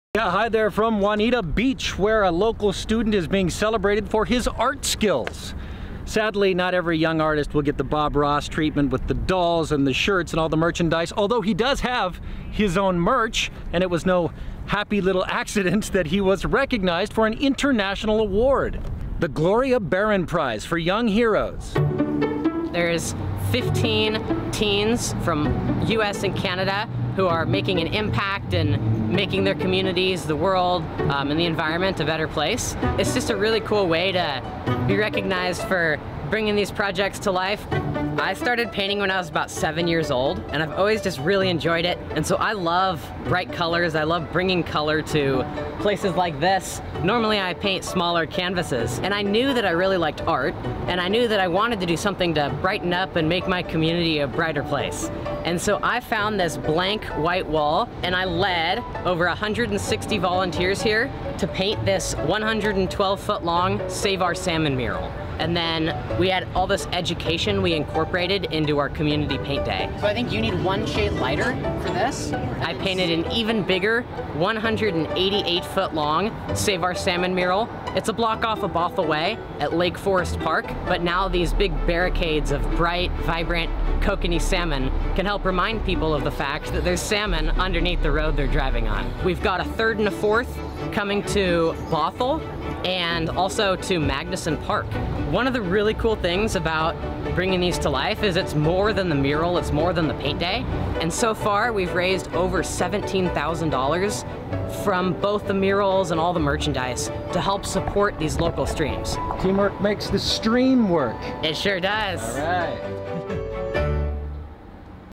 Young heroes prize recipient
A local high schooler's art-driven awareness campaign caught the attention of a national competition - earning him thousands to put toward higher education.

Juanita High School student Austin Picinich decided to use his passion for painting to create a project called Save Our Salmon Through Art (SOS) which aims to raise awareness about salmon restoration in north Lake Washington creeks. With the help of more than 160 volunteers, he has been able to transform blank walls into huge educational murals.

Picinich's first mural is 112 feet long near Juanita Creek in Kirkland.

“I drove by this big blank wall every day and thought this would be a perfect place to kick off my project,” Picinich said.

SOS has also painted a second mural in Lake Forest Park, which is 188 feet long. The mural straddles a roadway that crosses over McAleer Creek.

“Now people who drive that way will be reminded that there’s salmon in the stream running under that roadway,” Picinich said.

The project caught the attention of a national competition for young leaders.

Picinich is one of 15 recipients of the 2022 Gloria Barron Prize for Young Heroes. Executive Director Barbara Ann Richman said Picinich is a shining example of what the competition is looking for in young leaders.

“Austin was chosen for his Save our Salmon Through Art initiative, which creates vibrant public art projects and murals in the Greater Seattle area that engage, educate, and empower communities to restore salmon spawning streams. We love the way he’s using his artistic passion and talent for the greater good. His creativity, positivity, courage, and hard work are hugely inspiring, and we’re delighted to honor him!” Richman said.

The Save Our Salmon Through Art project already has plans to expand into Bothell and Magnuson park with even more locations in the works.